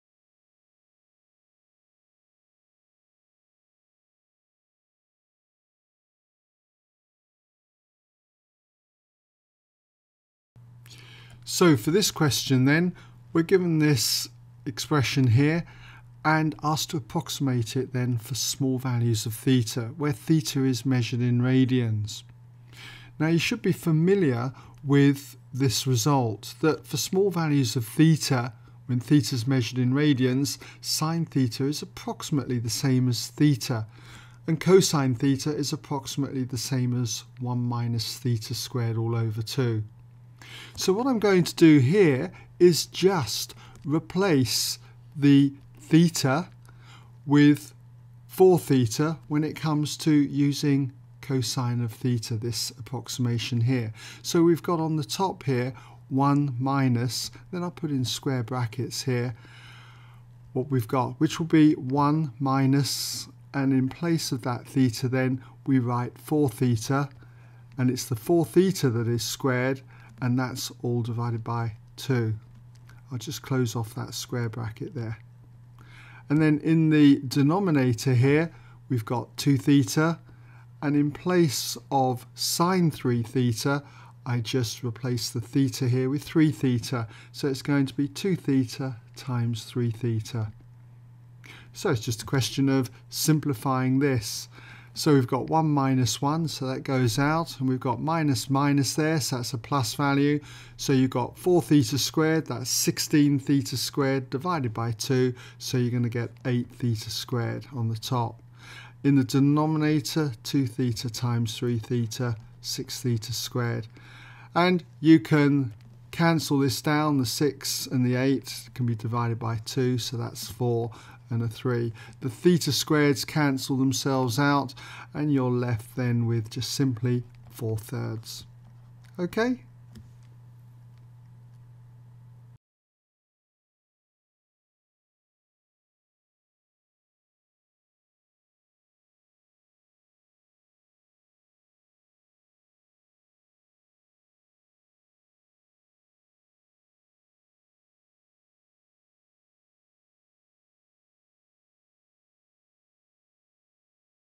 Edexcel | A-Level Pure Maths 2018 Paper 1 Q1 | ExamSolutions
Here you will find the worked solution to Edexcel A-Level June 2018 Pure Maths Paper 1 Question 1 on small angles.